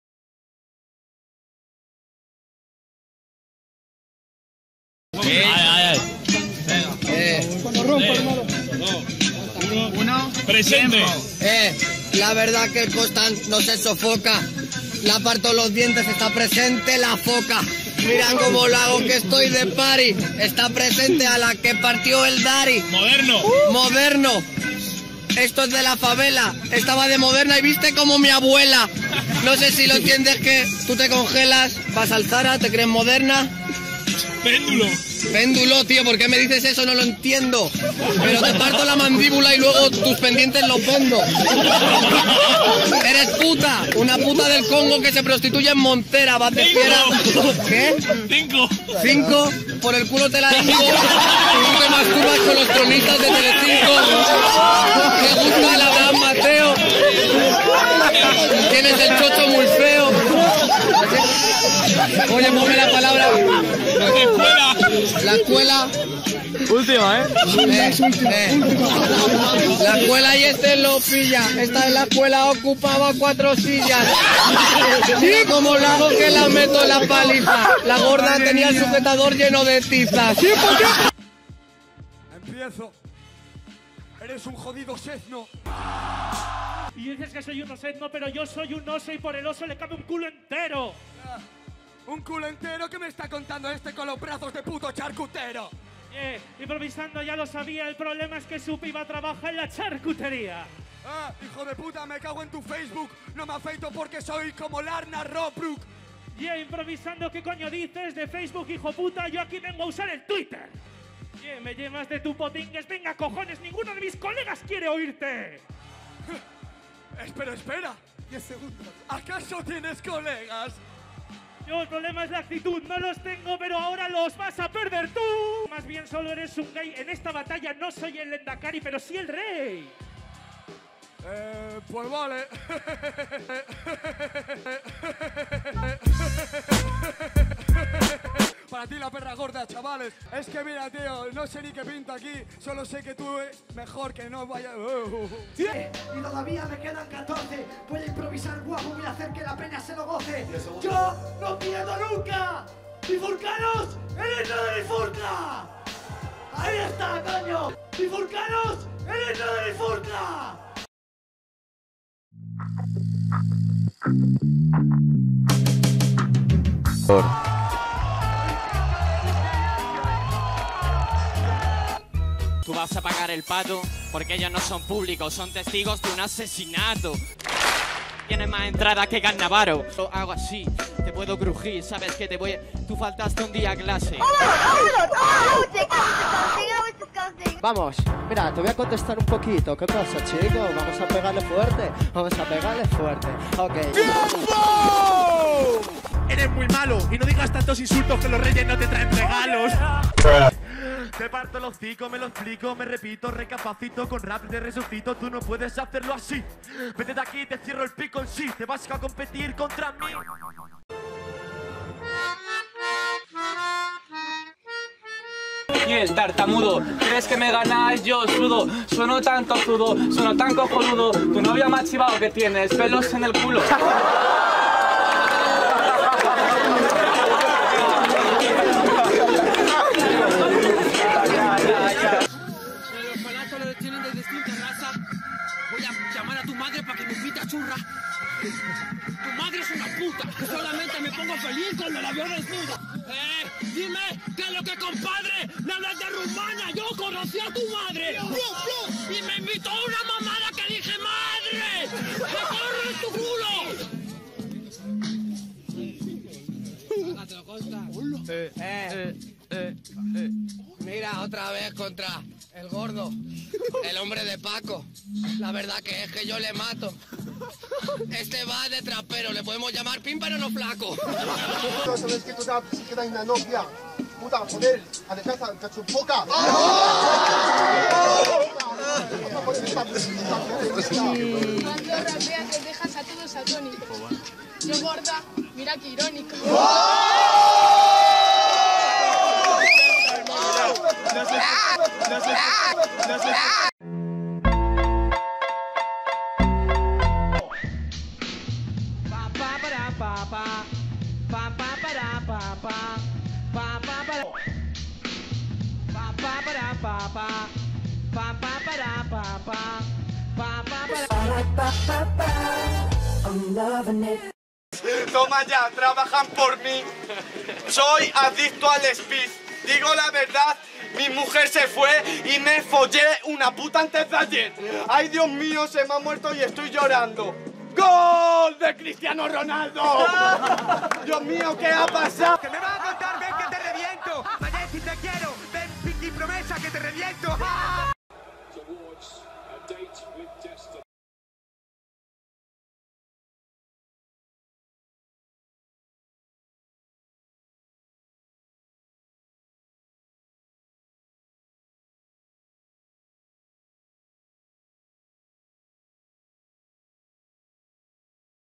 SI TE RIES PIERDES en BATALLAS de GALLOS (DIFÍCIL)
Si te ríes pierdes en Batallas de Gallos. Like y comenta si te gustó!
BATALLAS QUE SALEN EN EL VÍDEO POR ORDEN:
1. Kostan vs Erika
2. Reiro vs Neonath
3. Kapo 013 vs Spyderflow
4. Lopey vs Navalha
5. Chuty vs Sergiete
6. Arkano vs Serra
7. Bifo y Migi vs Bola y Enrik
8. Jado y Maurri vs Dary y Kostan
9. Garnett vs Gloria
10. Jandj vs Celedonio
11. Botta vs Impacto
12. Kowen vs "Zein"